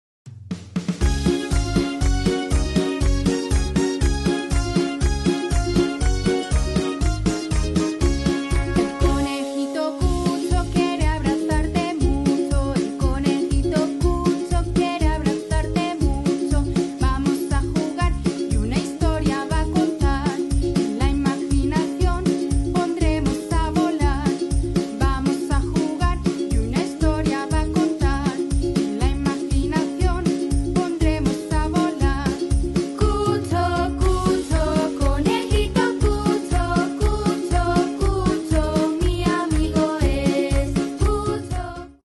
Cucho viajero en Sanabria
Pasar unos días de descanso es reparador.
Cucho está en el pueblo de Avedillo de Sanabria en Zamora🇪🇸
Visitando a sus primos ¡Qué bien se lo pasan juntos!